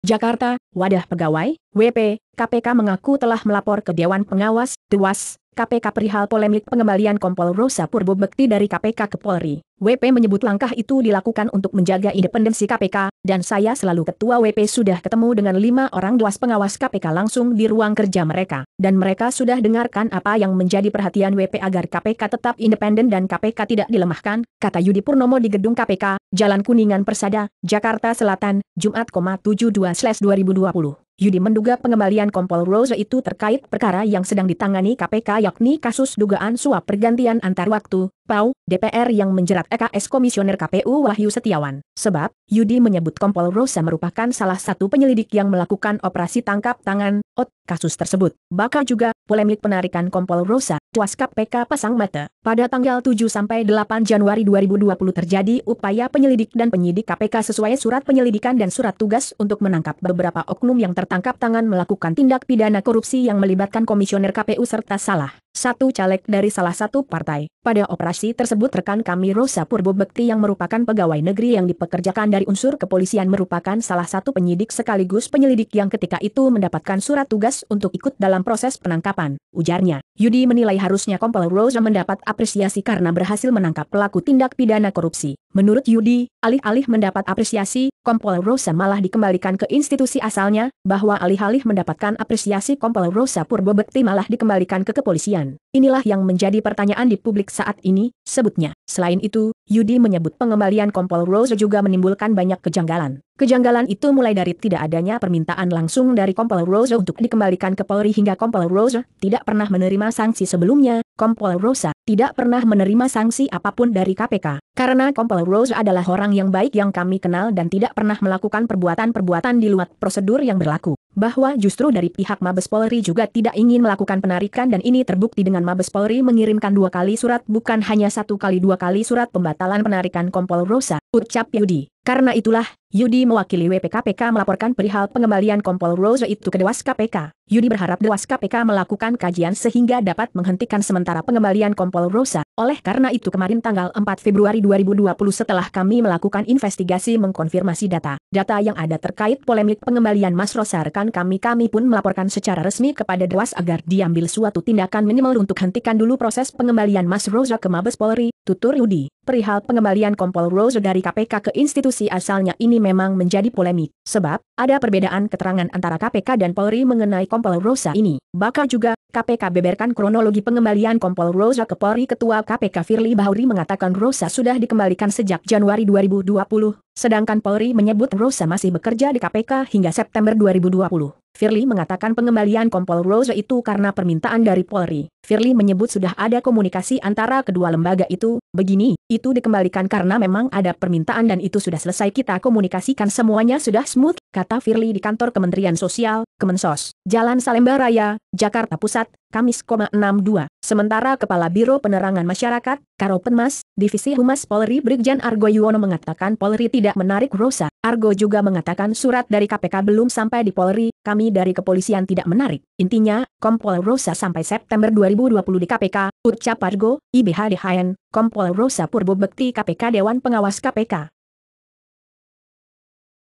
WP KPK Lapor Dewas Minta Pengembalian Kompol Rosa Dihentikan
Jakarta - Wadah Pegawai ,WP, KPK mengaku telah melapor ke Dewan Pengawas ,Dewas, KPK perihal polemik pengembalian Kompol Rosa Purbo Bekti dari KPK ke Polri. WP menyebut langkah itu dilakukan untuk menjaga independensi KPK. ,Dan saya selalu Ketua WP sudah ketemu dengan lima orang Dewas Pengawas KPK l